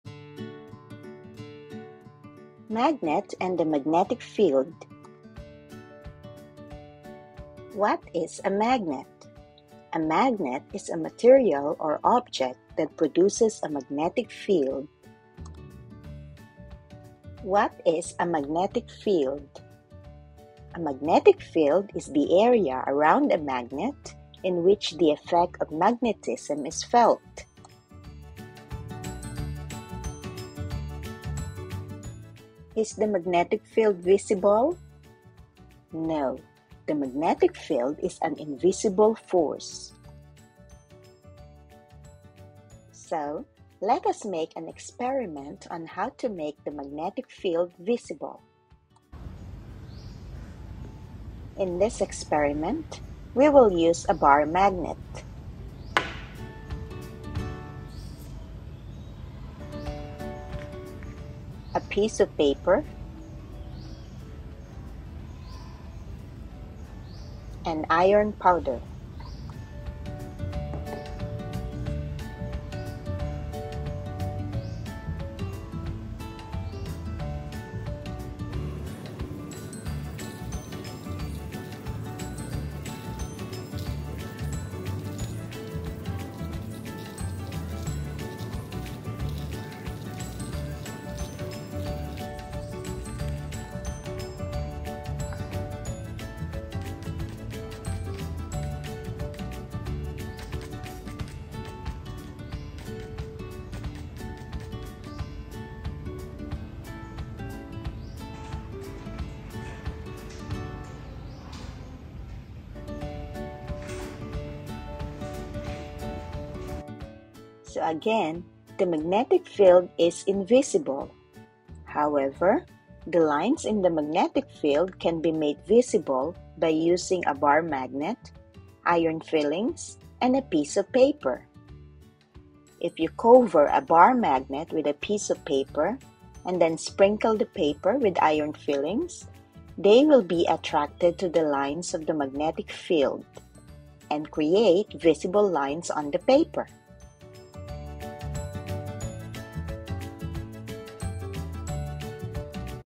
Magnet and Magnetic Field | How to Make the Magnetic Field Visible | Science Experiment
In this video, we will discuss about magnet and the magnetic field. We will also perform an experiment on how to make the magnetic field visible.

What is a magnet?
What is a magnetic field?
How to Make the magnetic field visible
Magnet & Magnetic Field